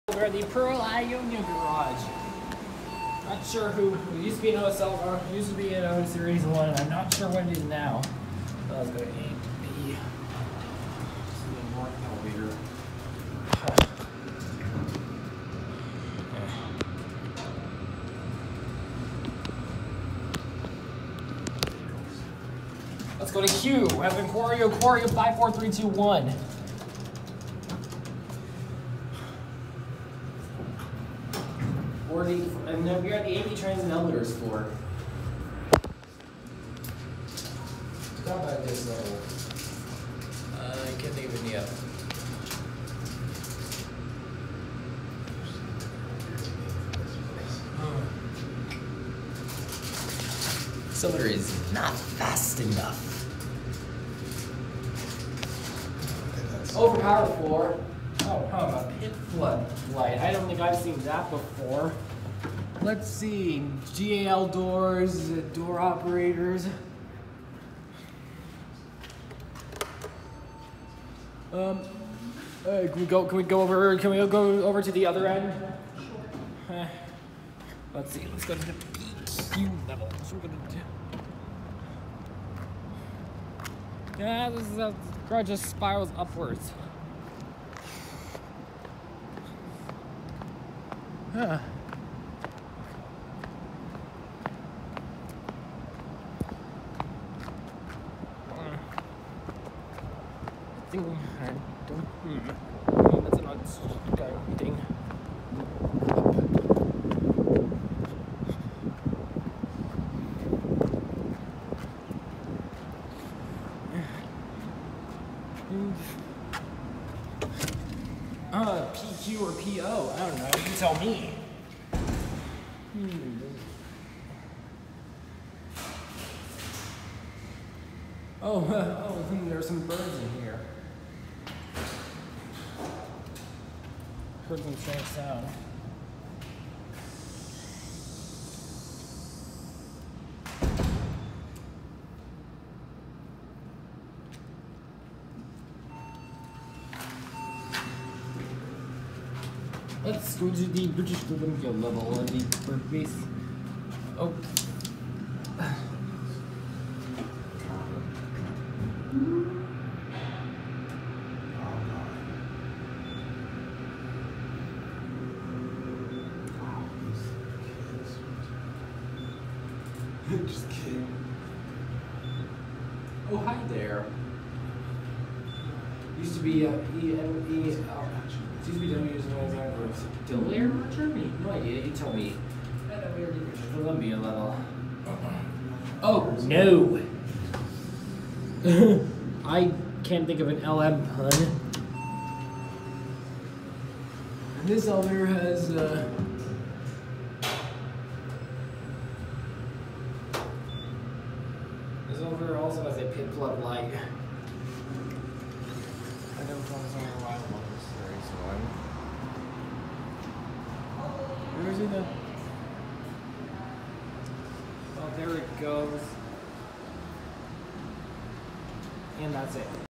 Modernized Otis Elevonic 401 Elevators @ Pearl/Ionia Garage, Grand Rapids, MI
You will learn how to pronounce my name. As well as some horrible puns. I bet you STL Elevators could start his own elevator company if he tried. And I bet you SirenTube can make better puns if he tried.